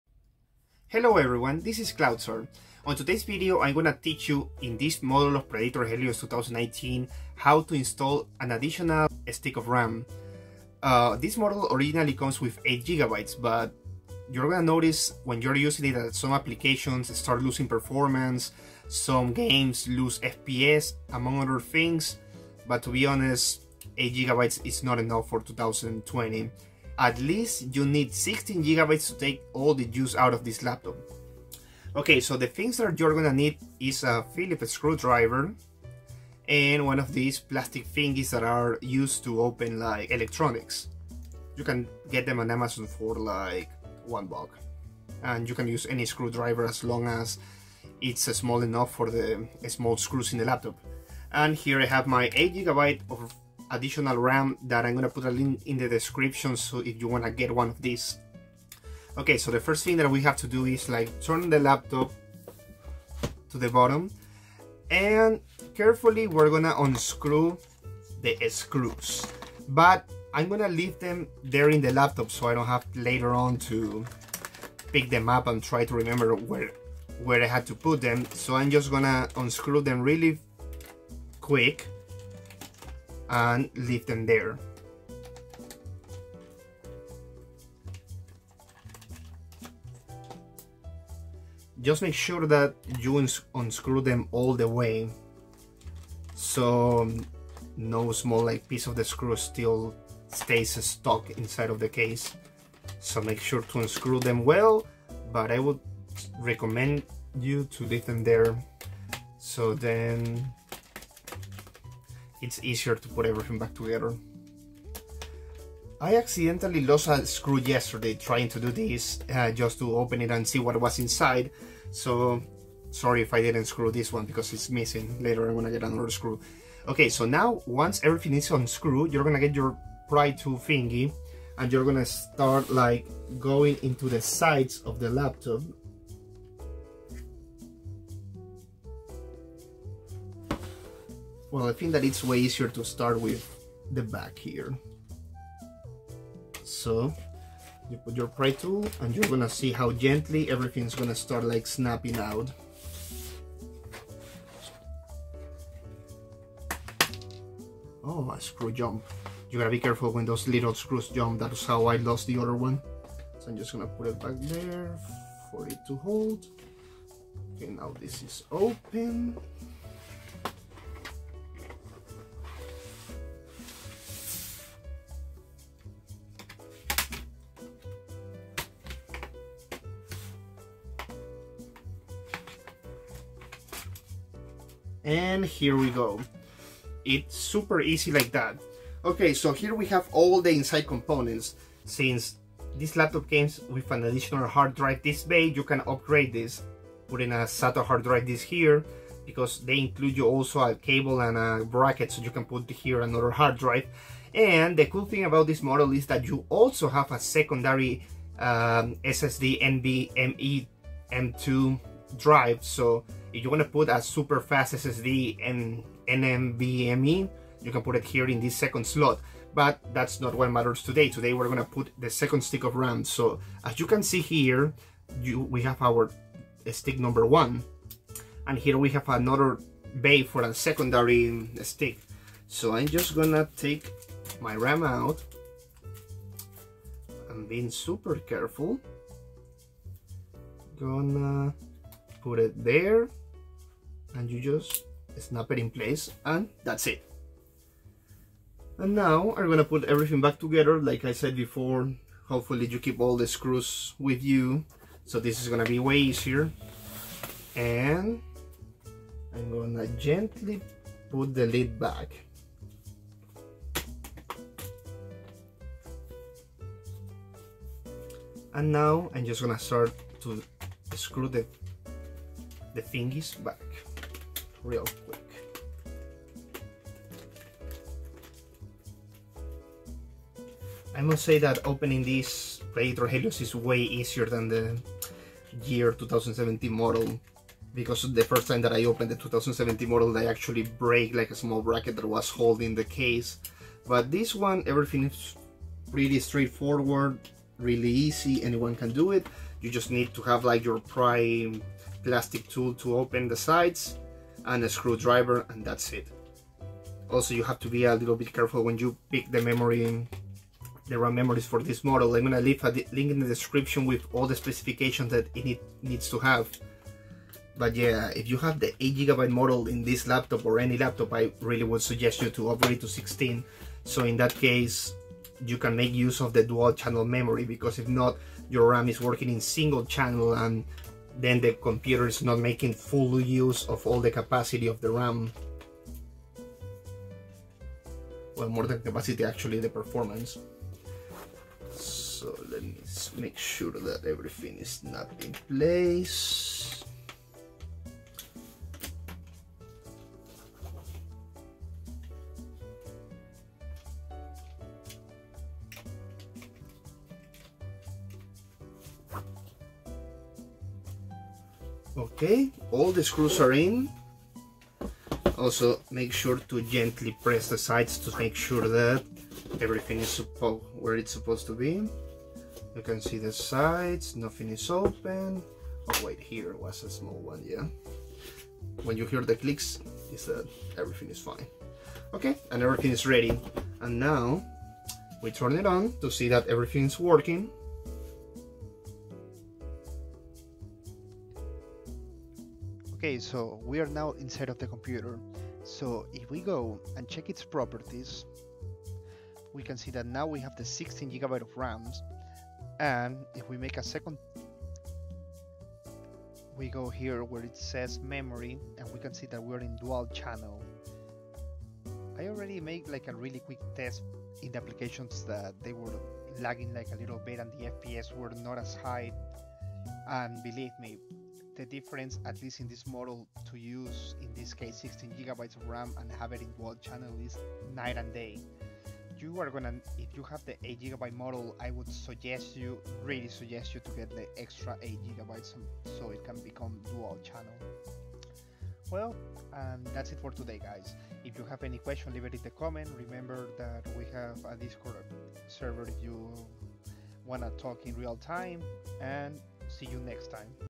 Predator Helios 300 i5-9300H 1660Ti Ram Upgrade
Adding extra 8 GB of DDR4 RAM to my Predator Helios PH315-52.

RAM used

Screwdriver kit

Pry tools